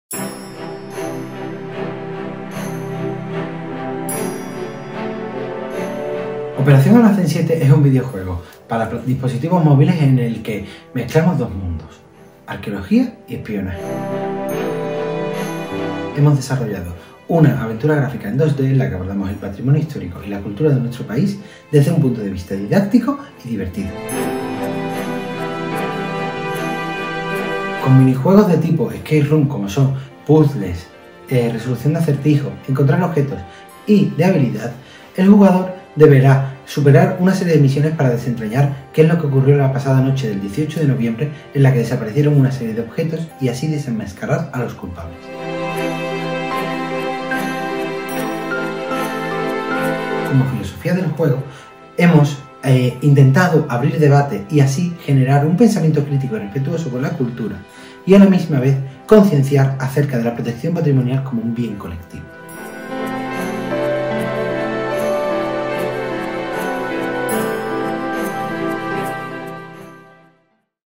Operación Almacén 7. Golmersa Servicios
Los mejores proyectos de Industrias Culturales en el Ministerio de Cultura y Deporte, 2021. Promoción del sector del videojuego y otras formas de creación digital.

Operación Almacén es un videojuego que convierte a los usuarios del mismo en agentes investigadores que tratan de descubrir la desaparición de ciertas piezas museísticas o artefactos arqueológicos de sus lugares de ubicación.

El evento “Los mejores proyectos de ICC en el Ministerio de Cultura y Deporte 2021” se celebrará el 08/11/22 a las 11h en el Espacio Fundación Telefónica de Madrid. Toda la información en este enlace